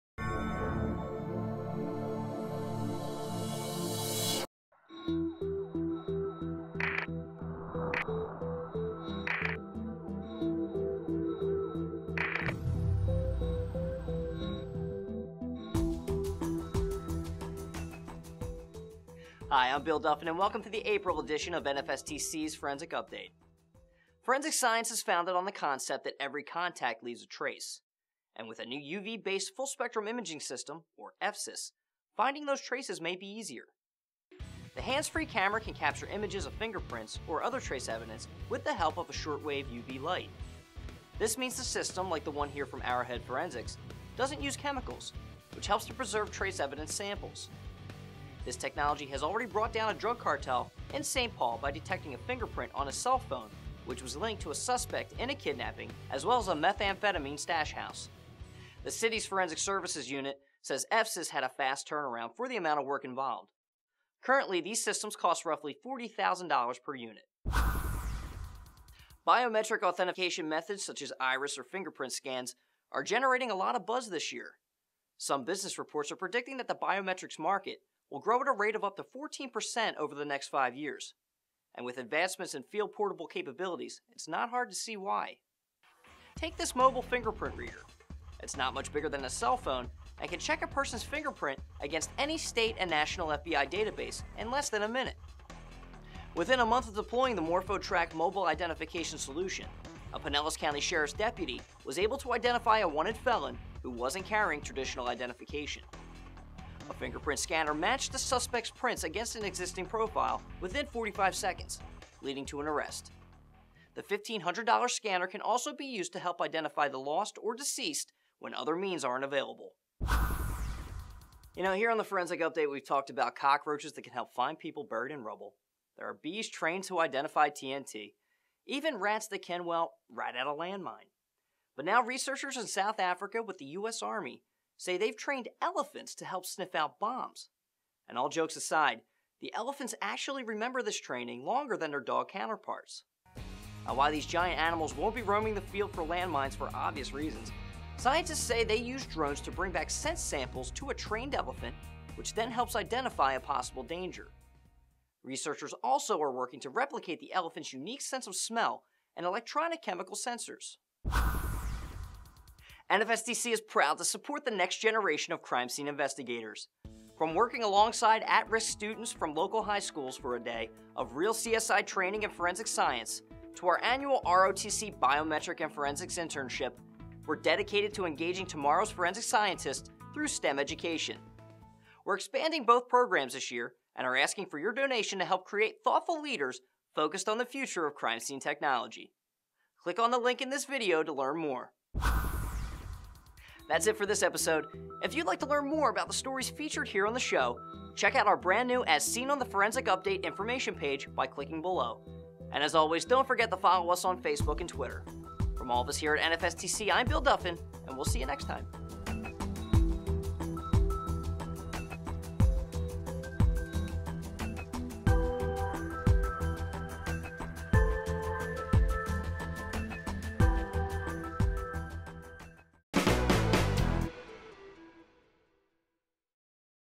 NFSTC | Forensic Update | April 2015
Featured in this edition: a new UV-based imaging system that can capture fingerprints; how elephants are being trained to assist the US Army, and more.  NFSTC’s Forensic Update brings you the latest in forensic science news from around the world.